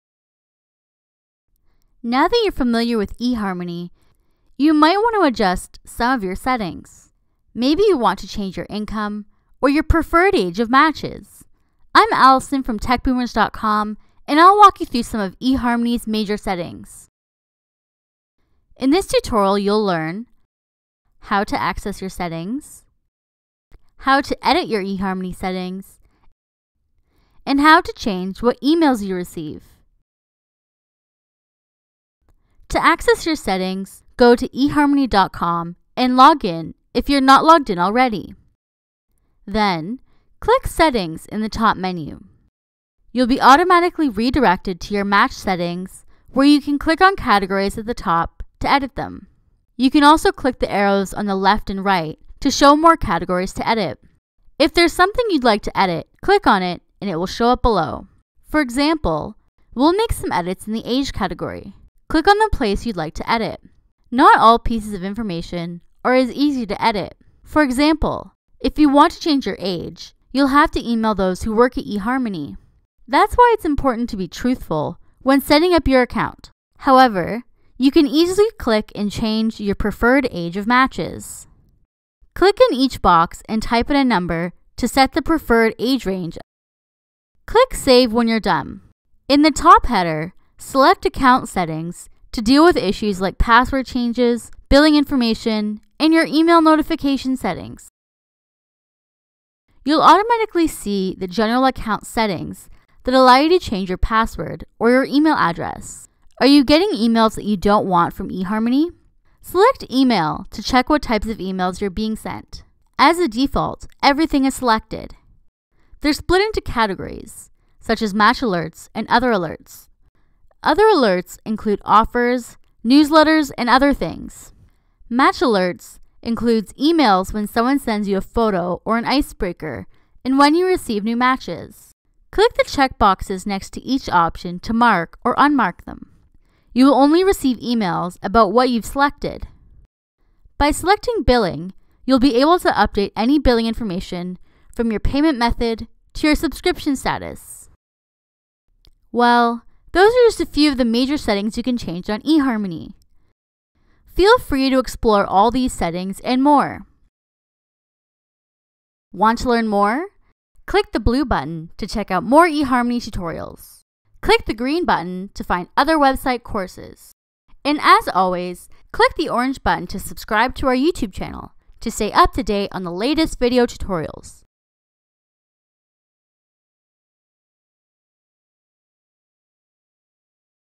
eHarmony Settings
We've set you up with a written version of this tutorial, in case you need it

Need help adjusting your settings on other websites? has the tutorials for you!

If you've decided that you're interested in a different kind of match, or just want eHarmony to stop cluttering your email inbox with alerts, we'll show you how to fix your settings.

In this tutorial, you will learn how to:

• Change your settings for what you're looking for in a match
• Choose when eHarmony will send you notifications via email
• Upgrade your account, and where to enter the billing information required for that